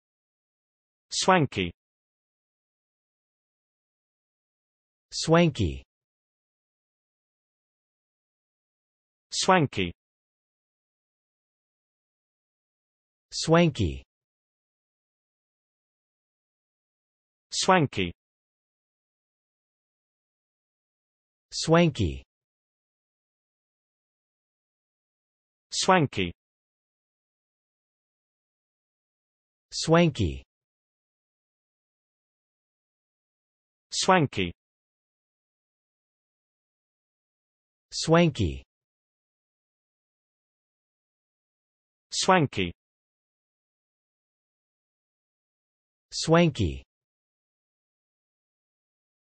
How to pronounce 'swanky' + meaning
swanky (adjective): imposingly fashionable and elegant. Synonyms: swank